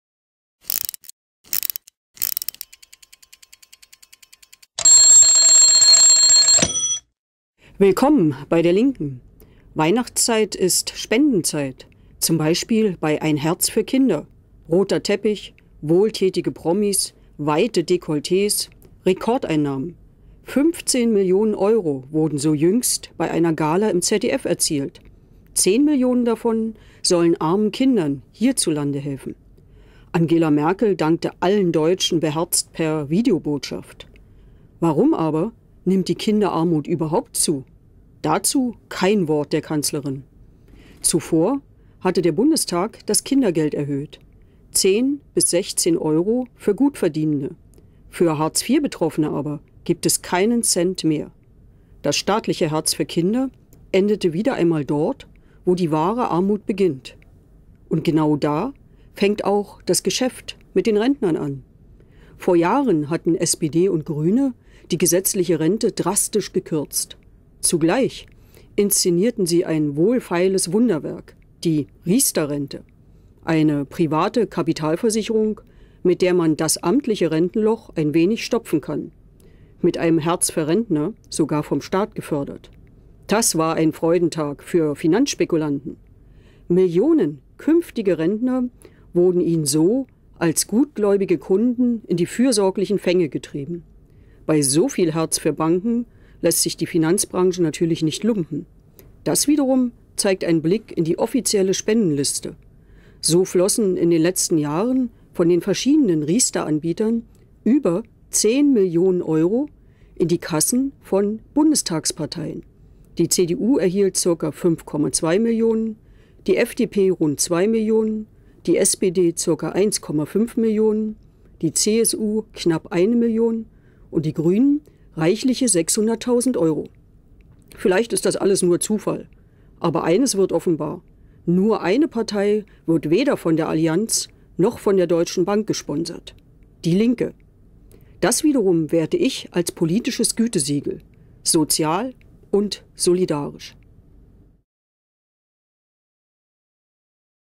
DIE LINKE: Petra Pau zu Spenden an Parteien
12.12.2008 - Versicherer, die die Riester-Rente anbieten, spendeten in den vergangenen 10 Jahren rund 10 Millionen Euro an die Parteien im Bundestag. DIE LINKE wird als einzige Partei weder von der Allianz noch von der Deutschen Bank gesponsort. Das ist als politisches Gütesiegel zu werten.